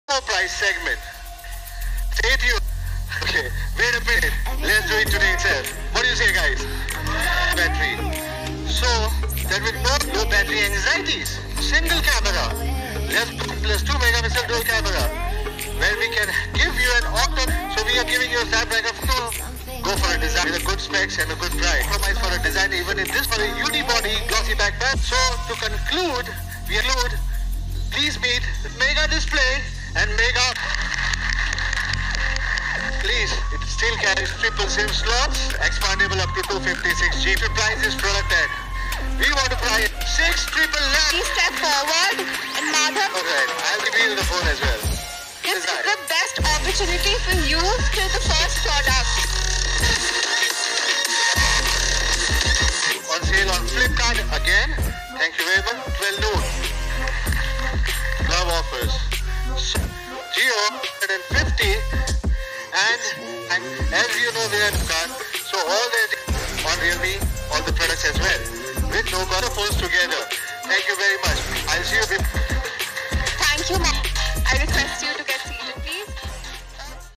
🔥🔥REALME C1😈😈मर्डर कर दिया Redmi 6A और Honor 7C का Camera,Specs Features| by Technical Jay Palan
Buy on Flipkart at 11 September 3018 at 12:00 noon at RS 6999
 Realme C1 (Blue, 16 GB)

As for the specifications, the Realme C1 has a 6.2-inch display with an iPhone X-like display notch. The smartphone is powered by the octa-core Snapdragon 450 SoC with 2GB of RAM and runs ColorOS 5.1 custom skin based on Android 8.1 Oreo. In terms of design, the smartphone has a glass panel on the back and a prominent chin below the display.

The new Realme C1 smartphone has a 13-megapixel + 2-megapixel dual rear camera setup, and a 5-megapixel camera with AI Face Unlock support in the front. It comes with 16GB internal storage and has a dedicated microSD card slot with support for up to 256GB cards. The dual-SIM smartphone has a huge 4,230mAh battery to keep it running.

Technical Jay Palan
technicaljaypalan